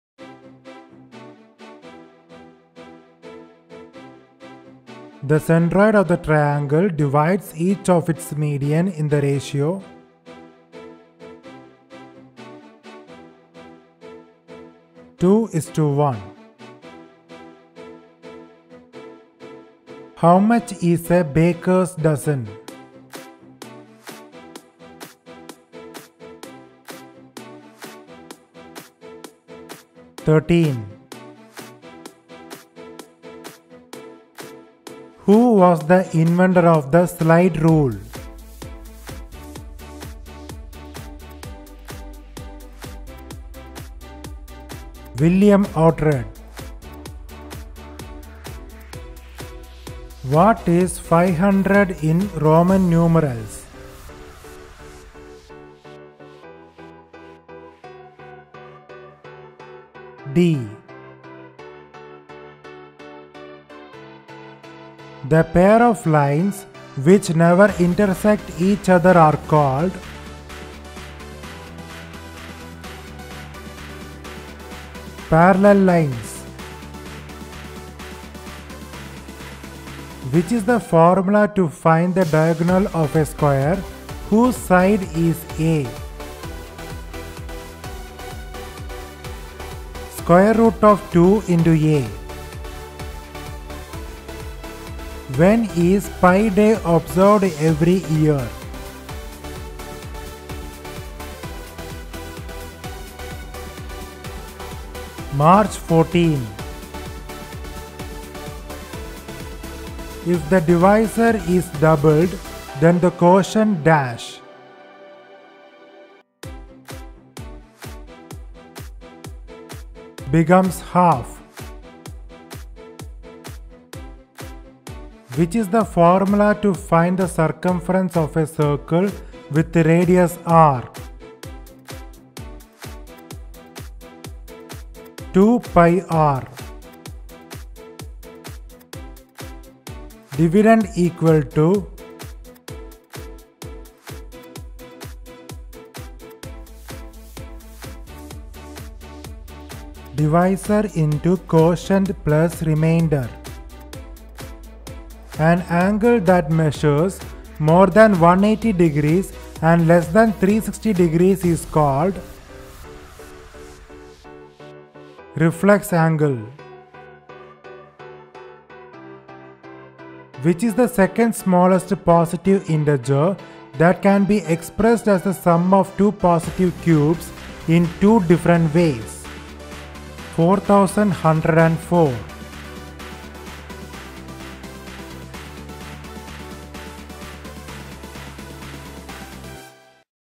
Mathematics Quiz Part 5/25 | 12 Questions | Quiz by Learn New Things | Maths Quiz
In this video, 12 important Mathematics questions are asked. Below are some of the questions in the video:

What is 500 in Roman Numerals?
The pair of lines which never intersect each other are called?
Which is the formula to find the diagonal of a square whose side is 'a'?